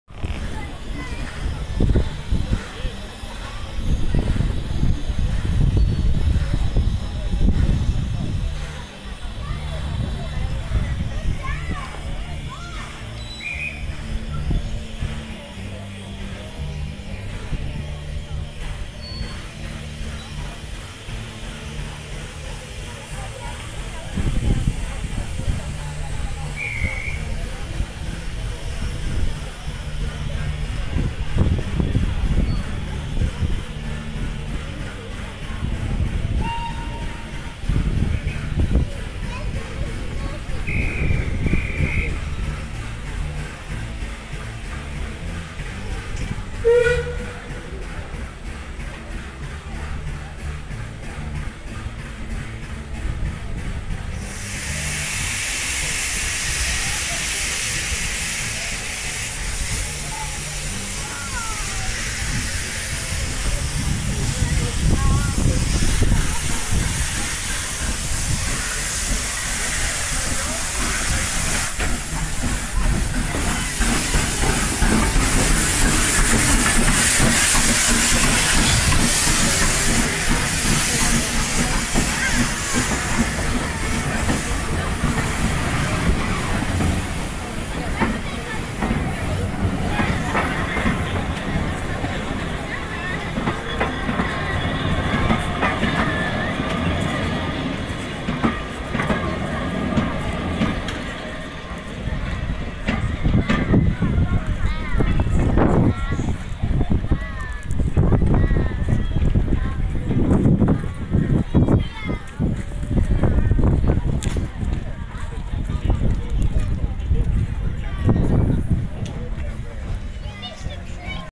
Isle of Wight steam railway havenstreet 2015
Steam train departing to smallbrook